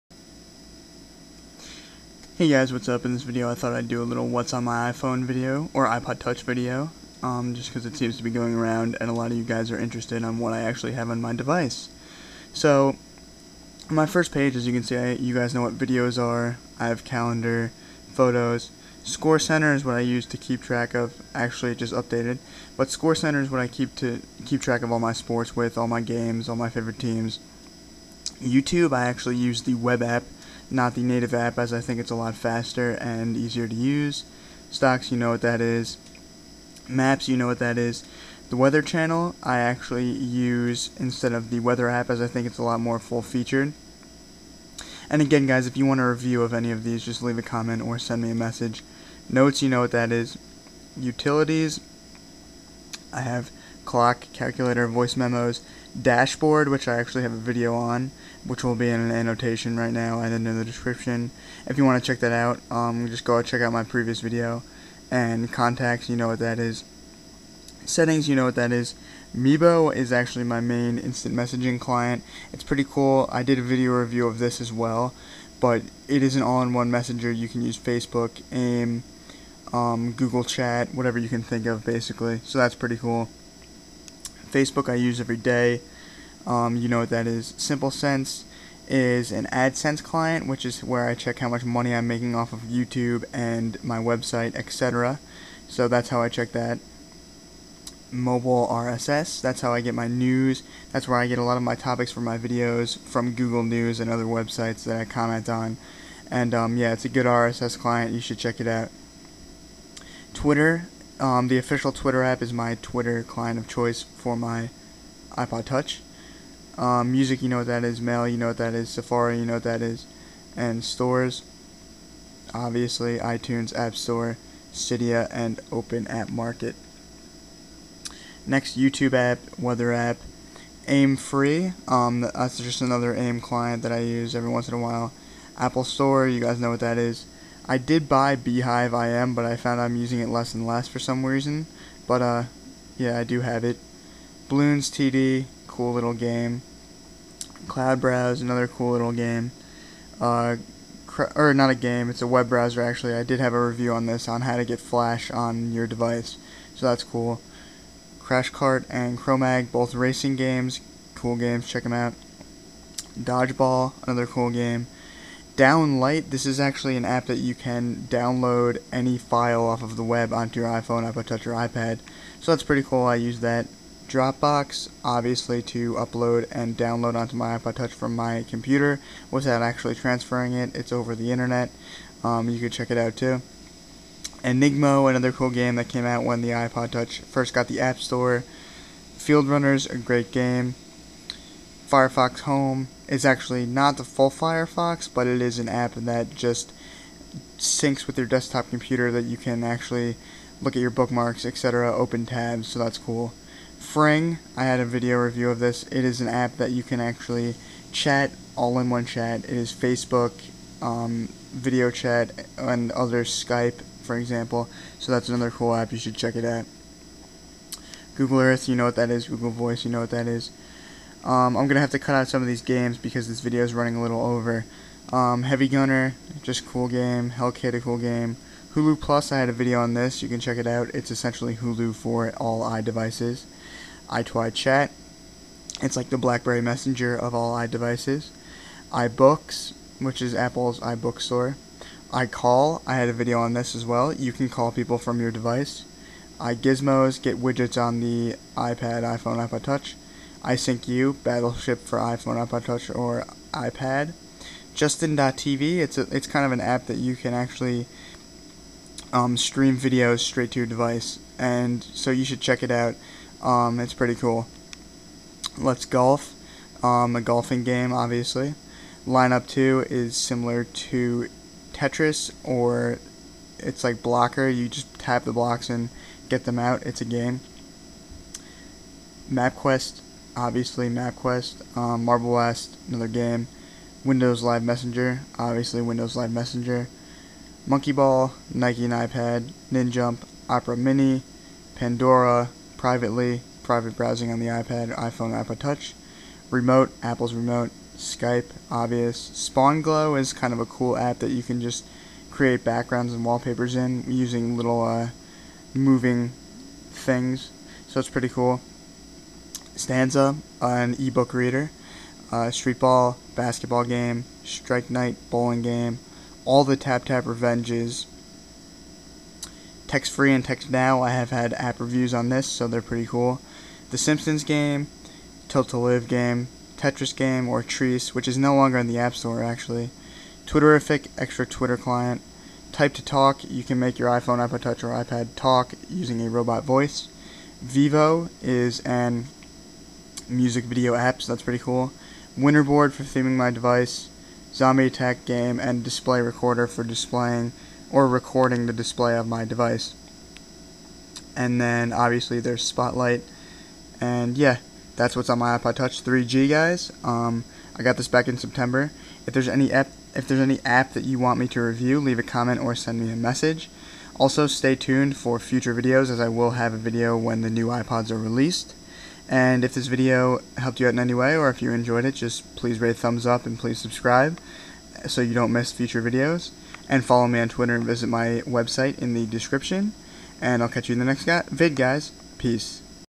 What's on My iPod Touch? My Favorite iOS 4 Applications
In this video, I give you an overview of the applications that are on my iPod Touch, and what some of my favorite iOS applications are at the moment. My iPod Touch (3G) is currently running 4.0 and is jailbroken. There are too many apps to list in the description, so check out the video for all of them! If you want a particular application reviewed, leave a comment or message me!